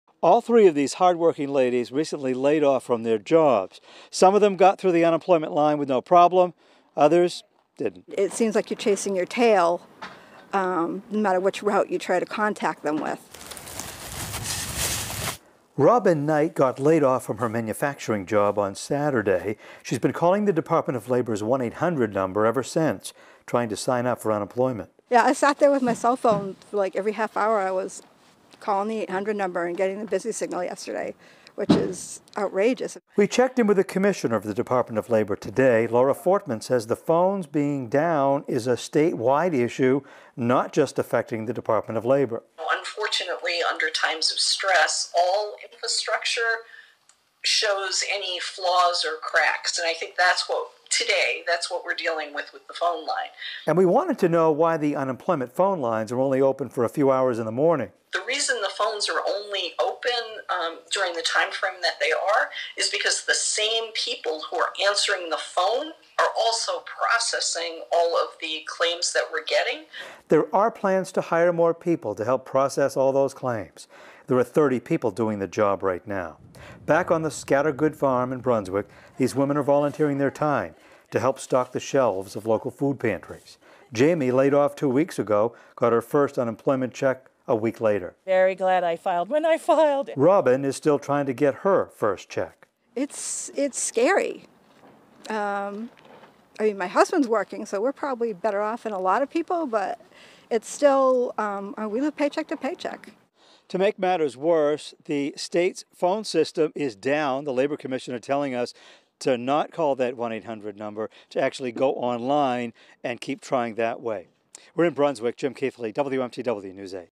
Laid-off workers having trouble filing for unemployment in Maine
Mainers are having trouble calling the 1-800 number, as well as issues getting on the state's website.